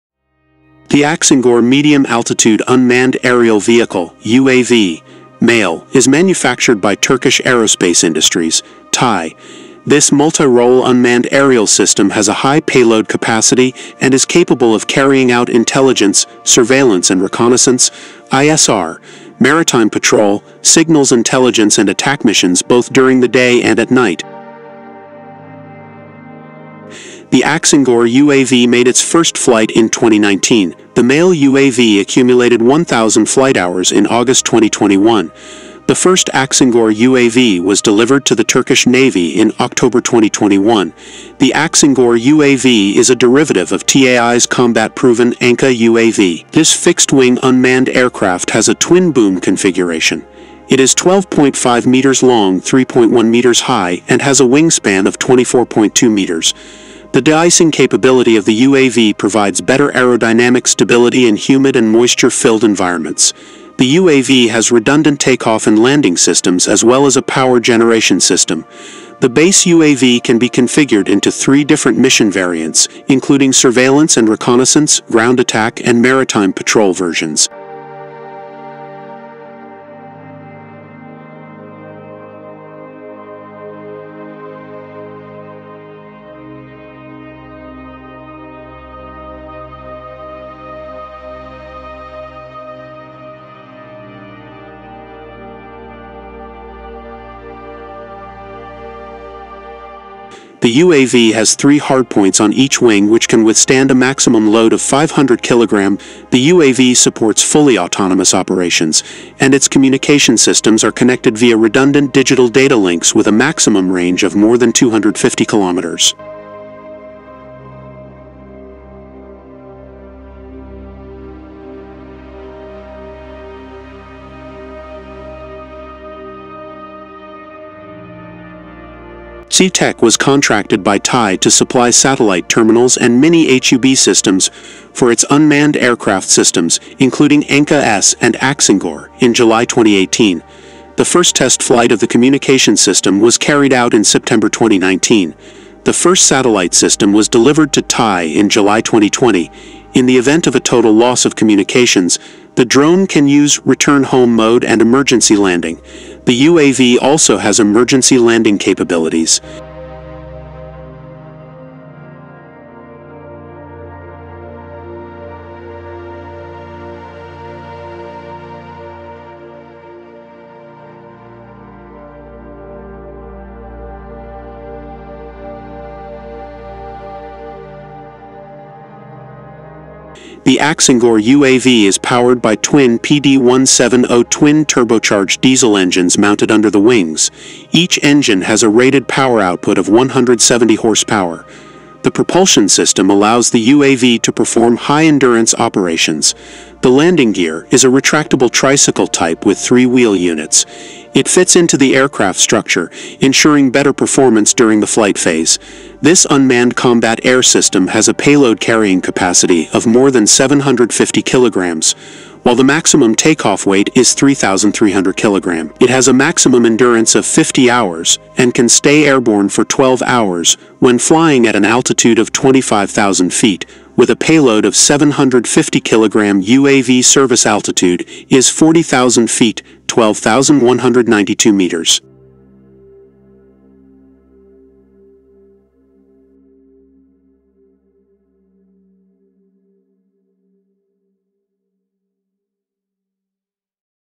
THE WORLD ADMIT !! This Türkiye's multi-role largest drone is the most difficult to defeat
This multi-role unmanned aerial system has a high payload capacity and is capable of carrying out intelligence, surveillance and reconnaissance (ISR), maritime patrol, signals intelligence and attack missions both during the day and at night.

The Aksungur UAV made its first flight in 2019. The UAV flew with 12 MAM-L ammunition for 28 hours at an altitude of 20,000 feet in September 2020.

The MALE UAV accumulated 1,000 flight hours in August 2021. The first Aksungur UAV was delivered to the Turkish Navy in October 2021.

The Aksungur UAV is a derivative of TAI's combat-proven Anka UAV. This fixed-wing unmanned aircraft has a twin-boom configuration. It is 12.5m long, 3.1m high and has a wingspan of 24.2m.

The de-icing capability of the UAV provides better aerodynamic stability in humid and moisture-filled environments. The UAV has redundant take-off and landing systems as well as a power generation system.

The base UAV can be configured into three different mission variants, including surveillance and reconnaissance, ground attack, and maritime patrol versions.

The UAV has three hardpoints on each wing, which can withstand a maximum load of 500kg.

The UAV supports fully autonomous operations, and its communication systems are connected via redundant digital data links with a maximum range of more than 250 km.

C-TECH was contracted by TAI to supply satellite terminals and miniHUB systems for its unmanned aircraft systems, including Anka-S and Aksungur, in July 2018. The first test flight of the communications system was carried out in September 2019. The first satellite system was delivered to TAI in July 2020.

In the event of a total loss of communications, the drone can use 'return home' mode and emergency landing. The UAV also has emergency landing capabilities.

The Aksungur UAV is powered by twin PD-170 twin-turbocharged diesel engines mounted under the wings. Each engine has a rated power output of 170hp. The propulsion system allows the UAV to perform high-endurance operations.

The landing gear is a retractable tricycle type with three wheel units. It fits into the aircraft structure, ensuring better performance during the flight phase.

This unmanned combat air system has a payload carrying capacity of more than 750kg, while the maximum take-off weight is 3,300kg. It has a maximum endurance of 50 hours and can stay airborne for 12 hours when flying at an altitude of 25,000ft with a payload of 750kg. UAV service altitude is 40,000 feet (12,192 m)